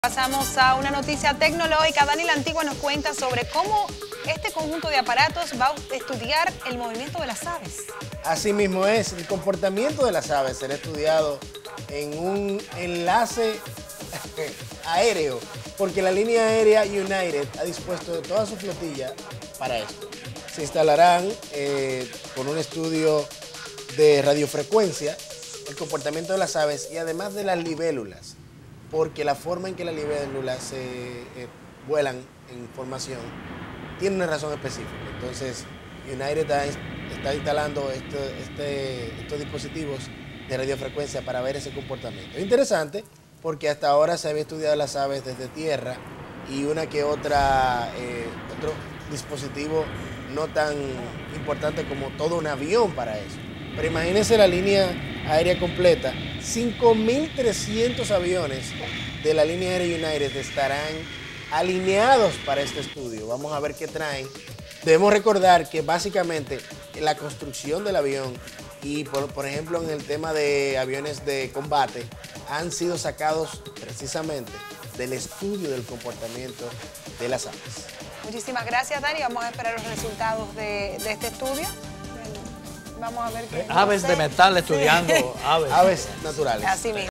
Toda una flota aérea para estudiar aves
United Airlines dispondrá de su 5,800 aeronaves para estudiar el comportamiento de las aves mediante sensores instalados en los aviones.